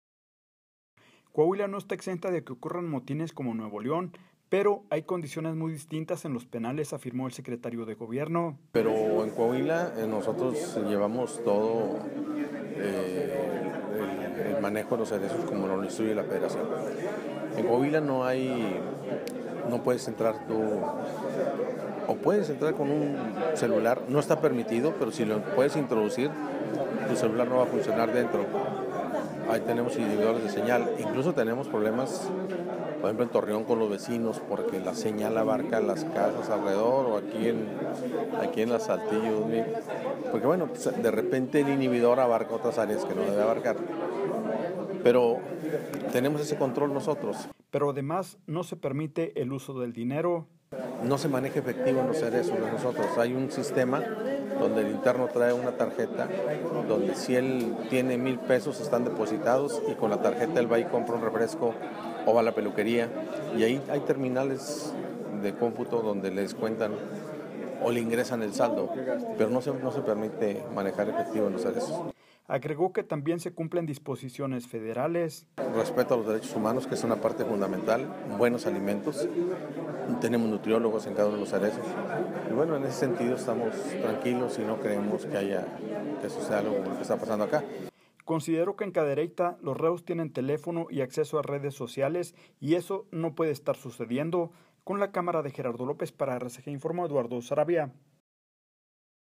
Descarta que haya motines en penales del Coahuila
Coahuila no está exenta de que ocurran motines como en Nuevo León, porque hay condiciones muy distintas en los penales, afirmó el Secretario de Gobierno.